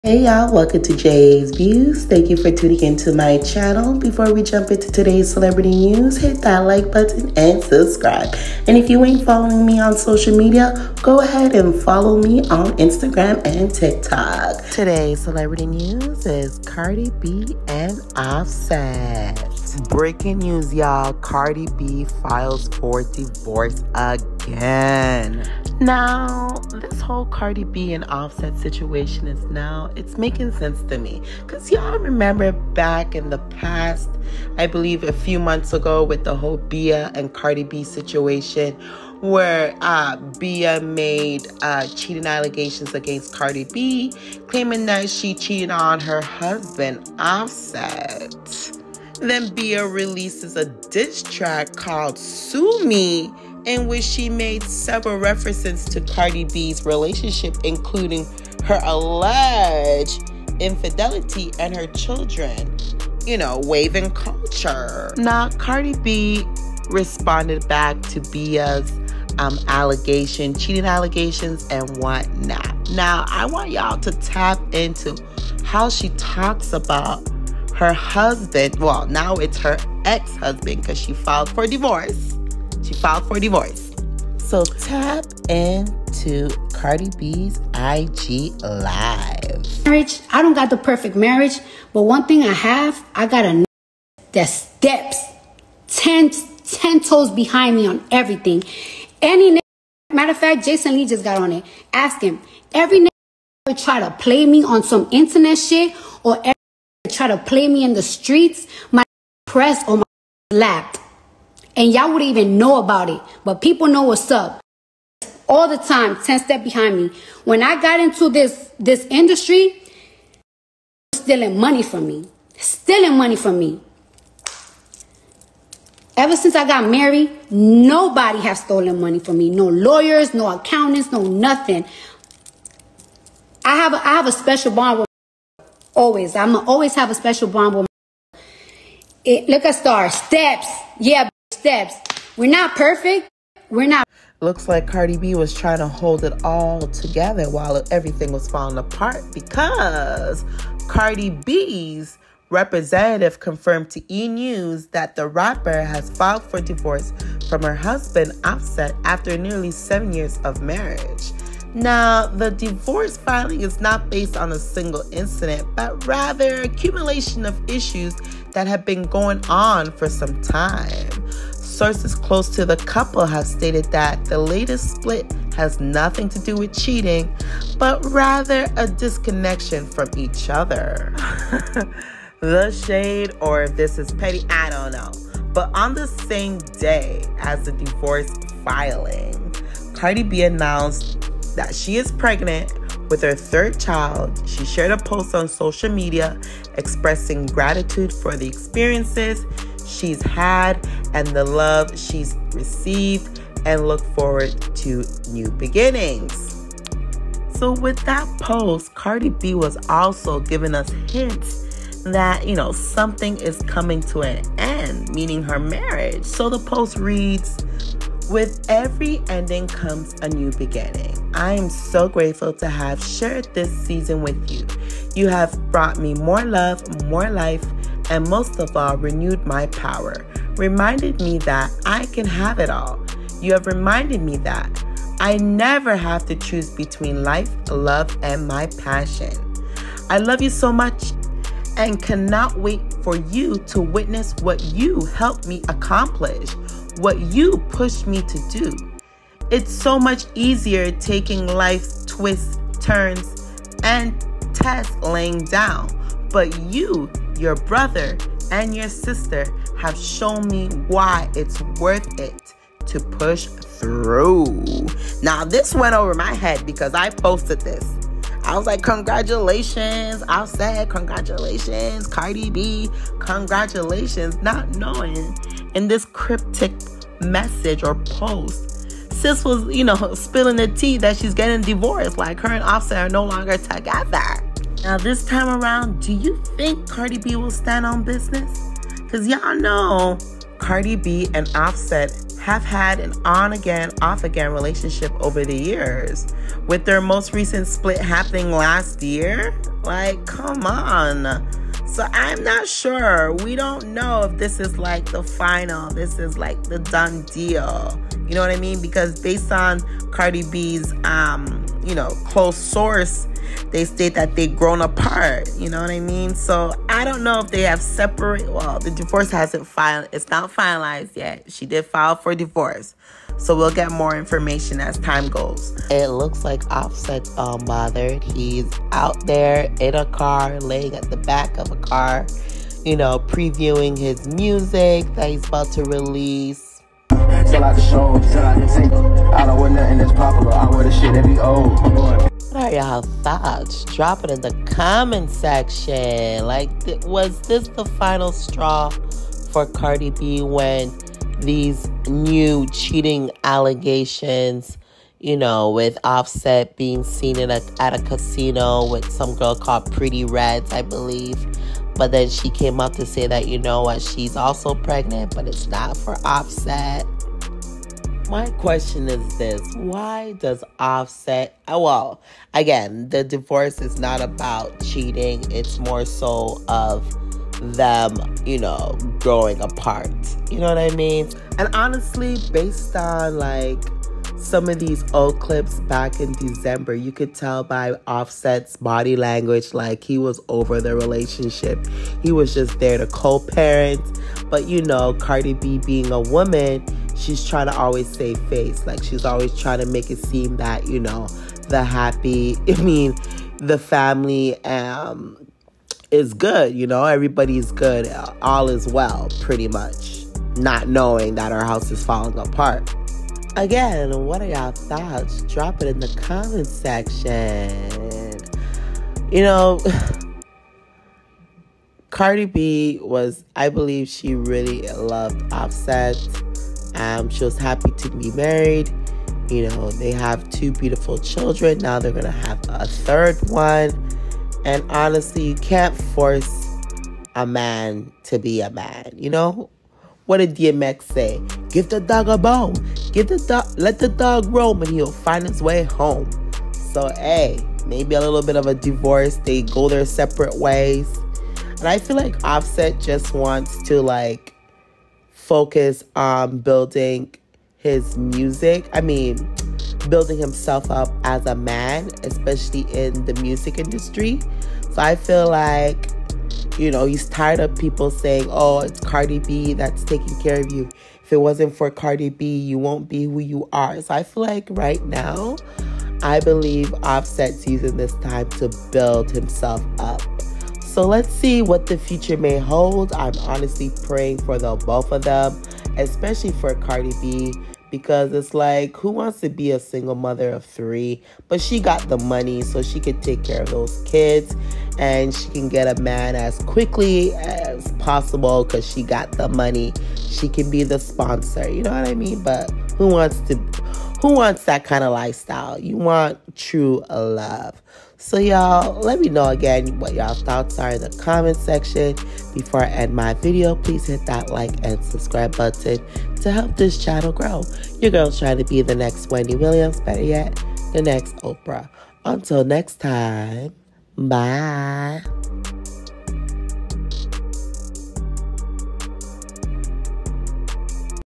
Cardi B Files for Divorce and Reveals Shocking Pregnancy News!
Cardi B drops two major bombshells: she's filing for divorce from Offset and expecting her third child!

Tap in hiphopculture